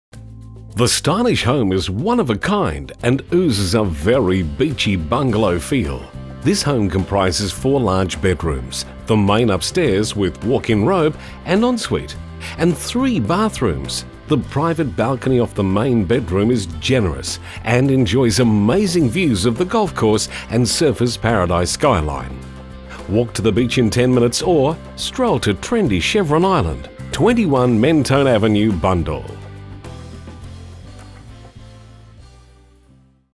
21 Mentone Avenue, Bundall QLD By Georgia Tritton
Property Video shoot of 21 Mentone Avenue, Bundall QLD by PlatinumHD for Ray White Benowa